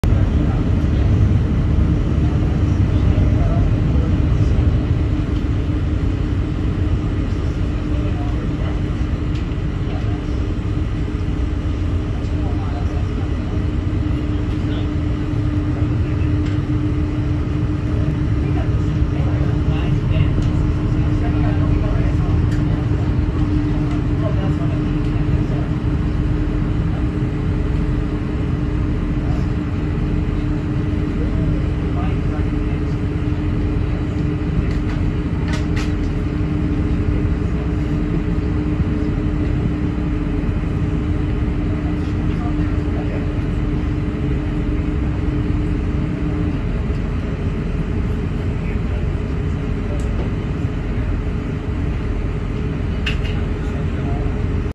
Ferry to Cumberland Island from downtown St. Marys, Georgia
￼ December is a great time to visit Cumberland Island national seashore off the coast of Southeastern Georgia. The ferry from downtown St. Mary’s Georgia goes to the island twice a day and takes 45 minutes. The trip recorded here left at 11:45 in the morning on Friday  December 8.